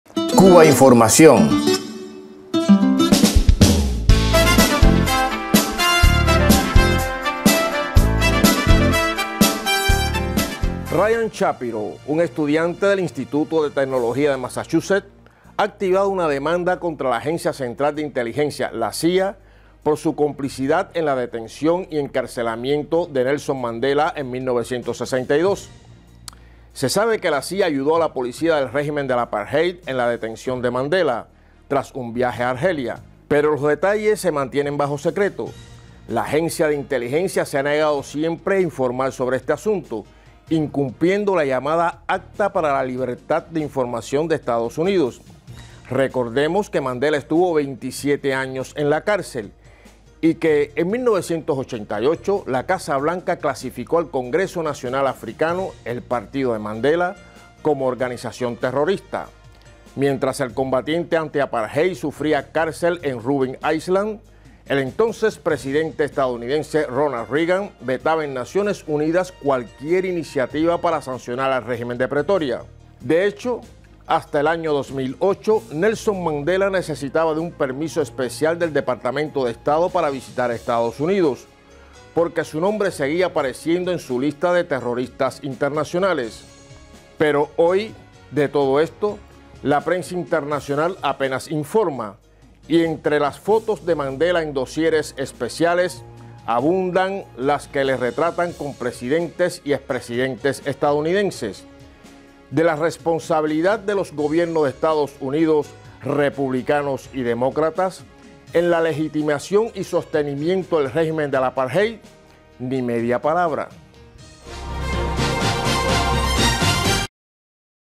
La CIA hizo que Nelson Mandela pasara 27 años en la cárcel: silencio informativo
Cubainformación TV -- Basado en un texto de Juan Manuel Karg - Sur y Sur / Blog "Isla mía" (Cuba).- Ryan Shapiro, un estudiante del Instituto de Tecnología de Massachusetts, ha activado una demanda contra la Agencia Central de Inteligencia (la CIA) por su complicidad en la detención y encarcelamiento de Nelson Mandela en 1962.